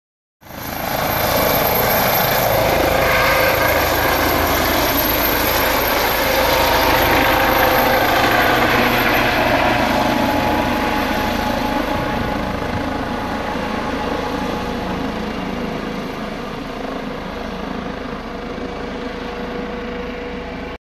Rettungshubschrauber Christoph 20 startet in Bayreuth
ADAC Rettungshubschrauber Christoph 20 startet in Klinikum Bayreuth